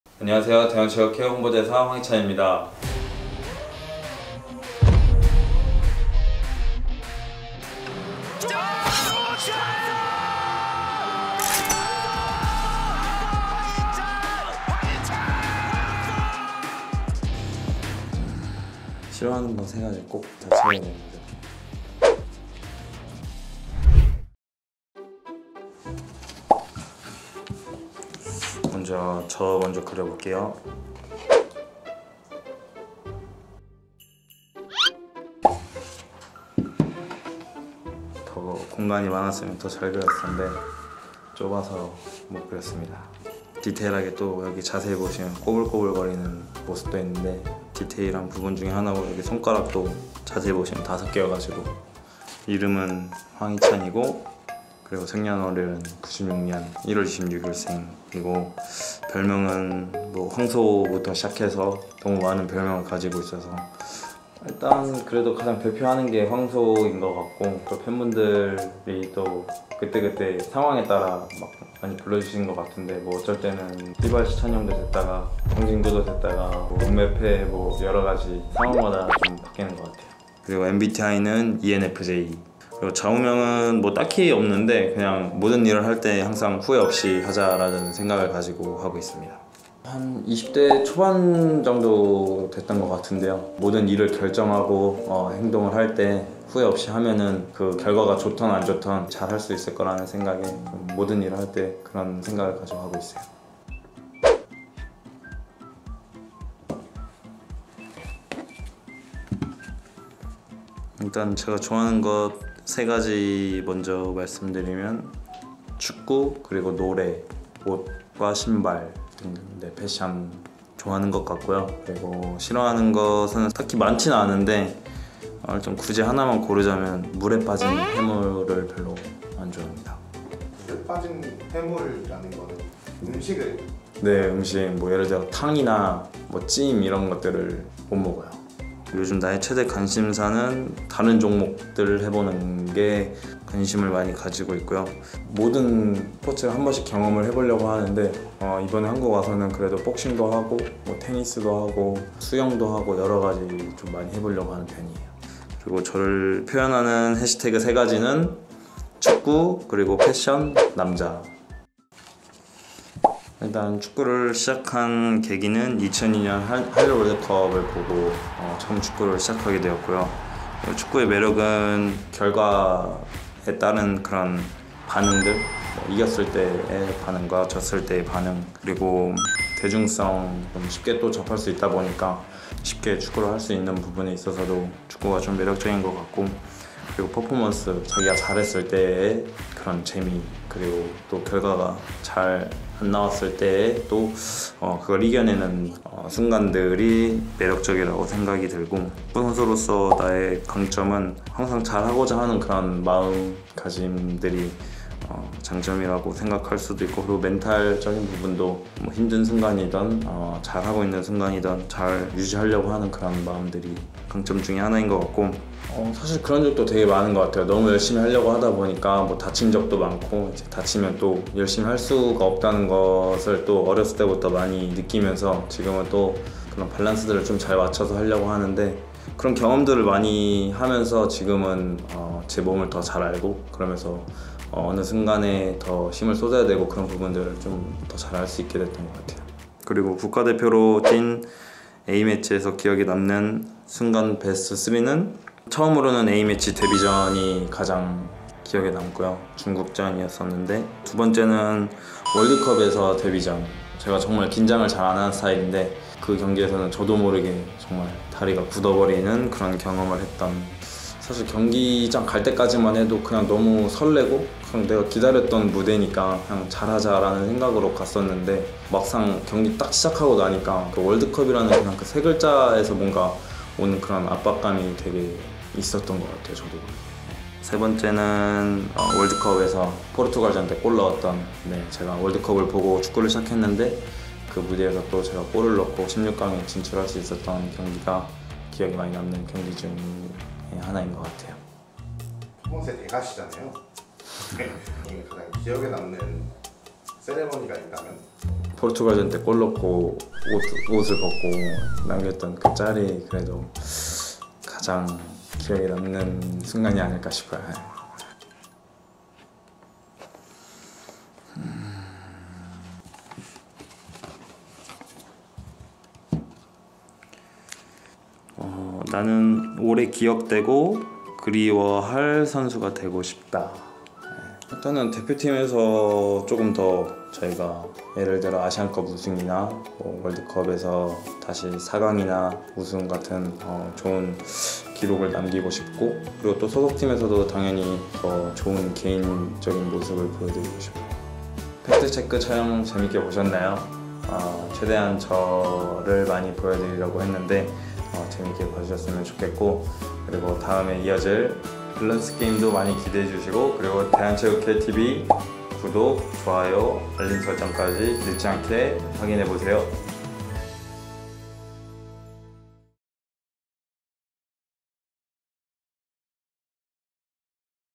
’The KOREAN Guy’ 황희찬이 대한체육회에 떴다…?!황희찬이 직접 쓰는 ✅팩트체크✅
대한체육회 홍보대사 황희찬과 진행하는 ✅팩트체크✅
황희찬이 작성한 자기소개서…? 이런 걸 볼 수 있다니 완전 럭키비키잖앙🍀
대한체육회tv에서만 볼 수 있는 황희찬st 자기소개서 보러가시죠!

*밸런스게임 당첨자는 다음 편에서 공개됩니다!!

*개인적인 비방 및 욕설 등의 댓글은 삭제될 수 있습니다.
*본 콘텐츠는 국민체육진흥기금 지원을 받아 제작되었습니다.
*대한체육회는 청렴하고 공정하게 업무를 수행합니다.